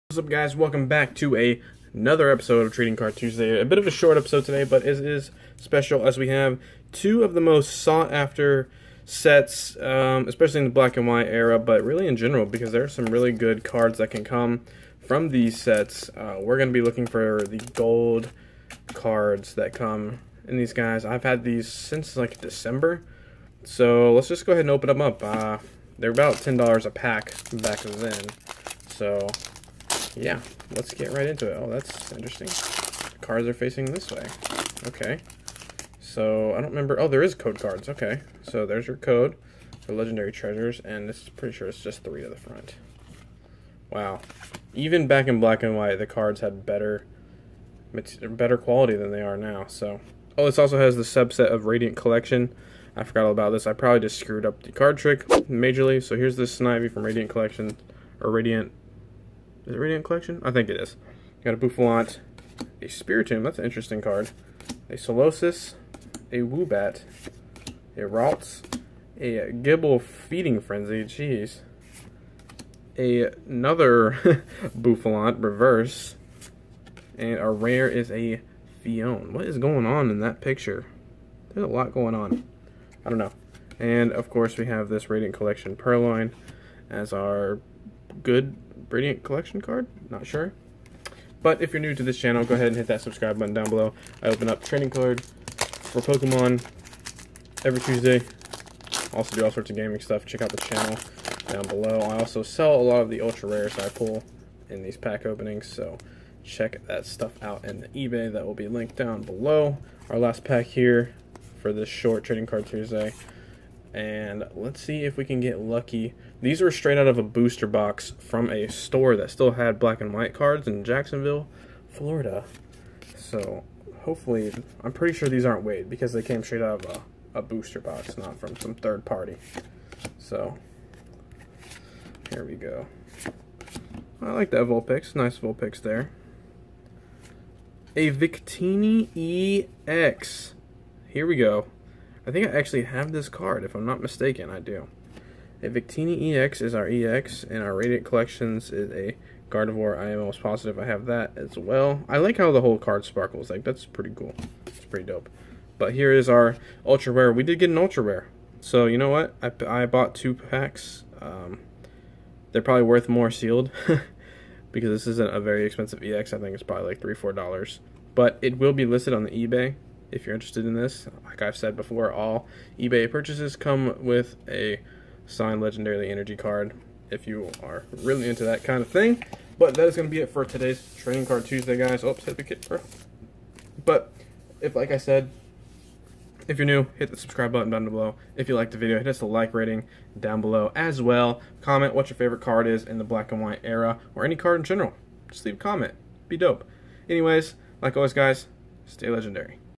SEARCHING FOR *GOLD* CARDS!!! LEGENDARY TREASURES OPENING - TCT #9
Legendary Treasures is a great set! Can we find the popular gold card?